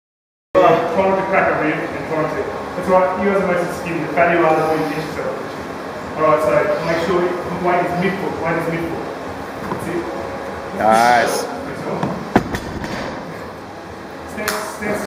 Toni Deadlifts 130kg w/Belt
At the Powerlifting coaching course